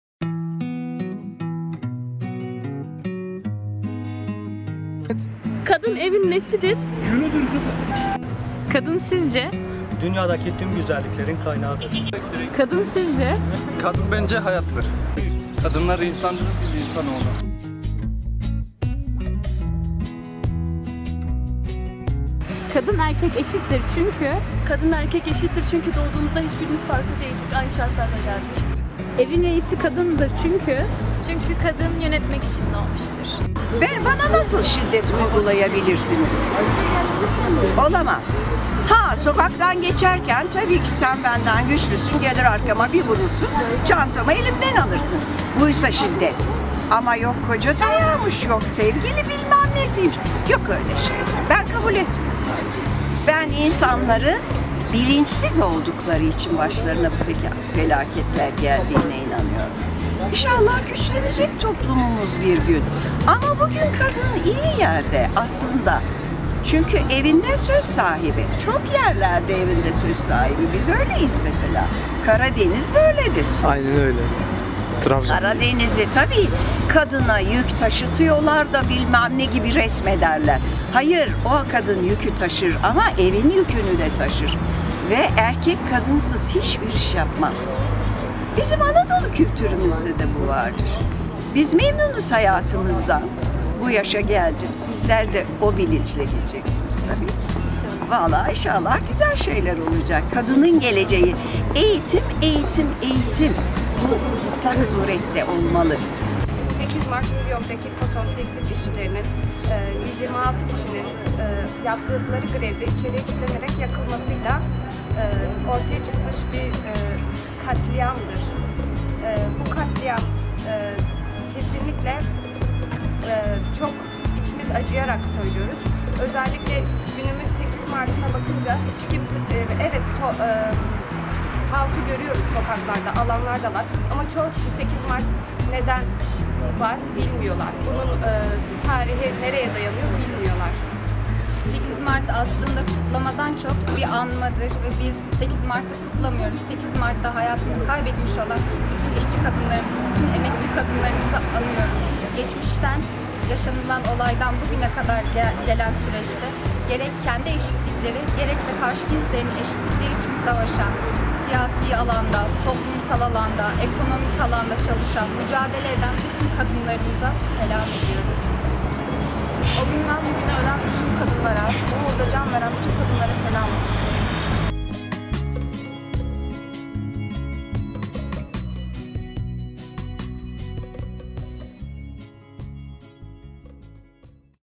Kadınlar Günü Sokak Röportajı - Erzincan Üniversitesi TDGK
Ayrımcılığa karşıyız;
Önce insan, sonra kadınız!